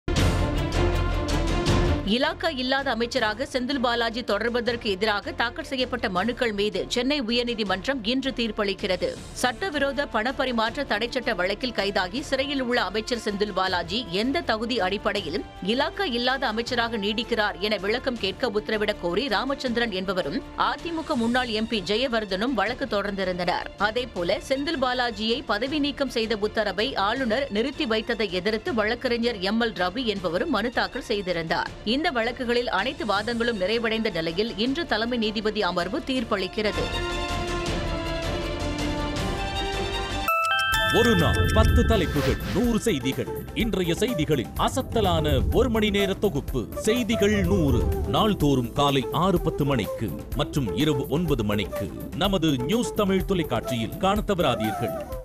அமைச்சராக தொடர்வாரா செந்தில் பாலாஜி? இன்று வெளியாகும் மிக முக்கிய தீர்ப்பு | Senthil Balaji case
அமைச்சராக தொடர்வாரா செந்தில் பாலாஜி? இன்று வெளியாகும் மிக முக்கிய தீர்ப்பு | Senthil Balaji case | DMK |  NewsTamil24x7